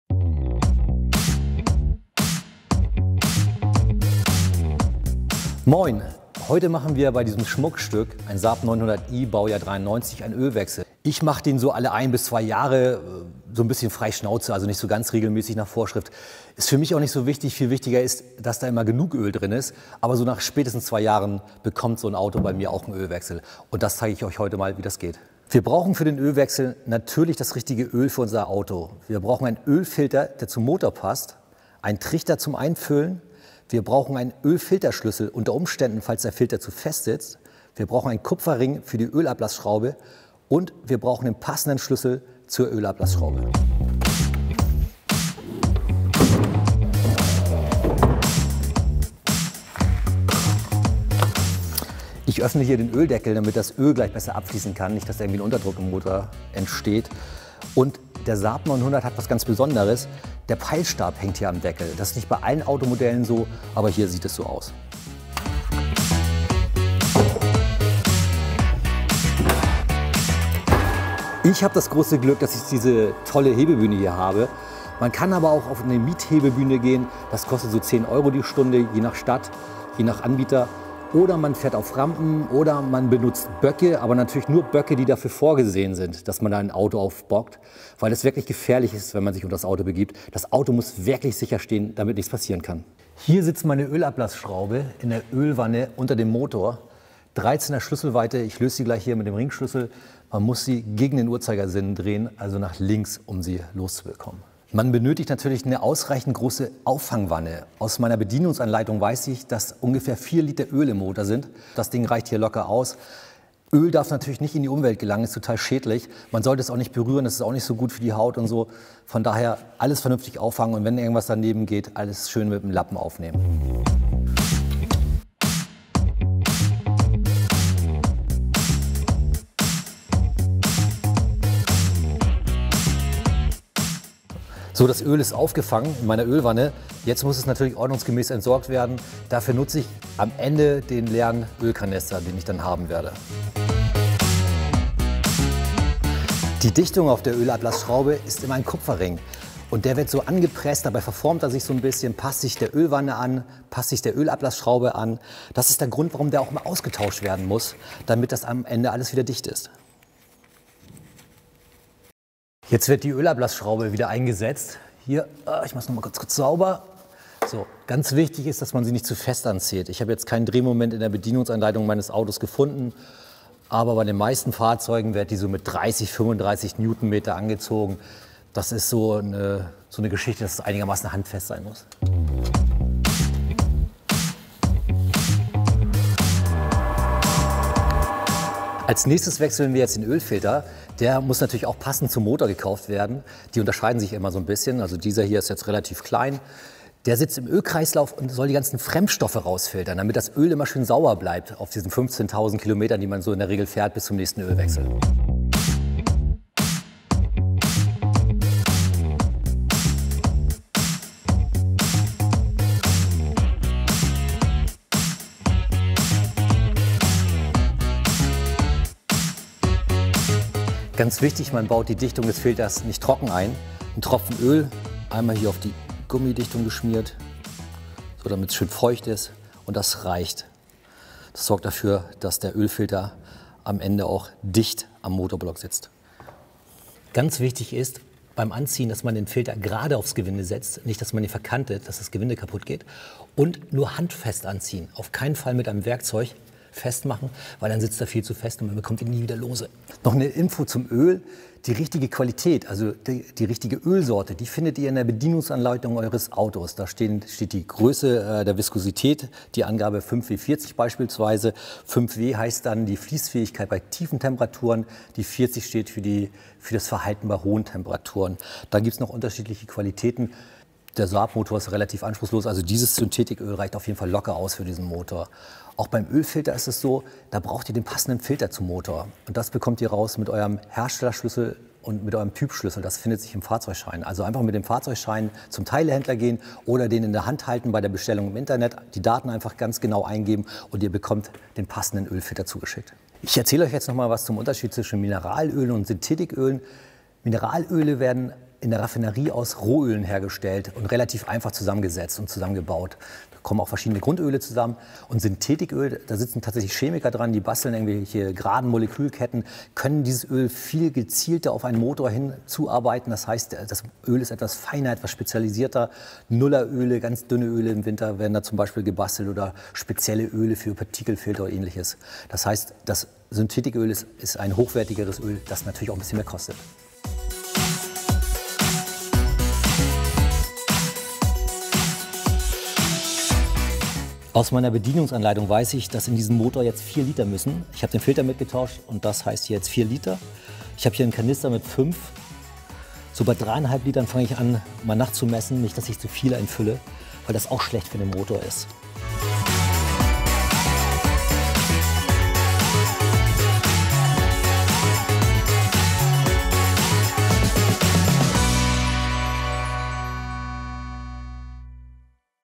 So helfe ich mir selbst (2017) - Das Ölwechsel 1x1
Um dem Motor ein langes Leben zu bescheren, sollte das Öl regelmäßig gewechselt werden. AUTO BILD zeigt, wie der Ölwechsel funktioniert und worauf zu achten ist.